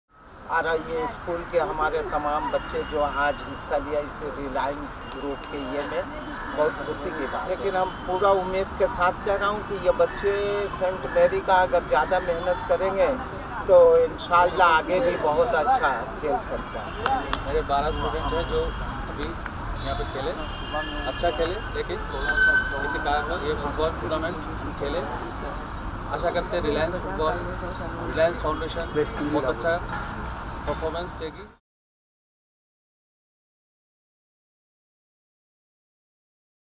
RFYS: Kolkata Jr. Boys - Nehalin day junior high School & St. Mary's school Interview
Watch the interview of Nehalin day junior high School & St. Mary's school Coaches after their RFYS: Kolkata Jr. Boys match held on 10th November, 2016.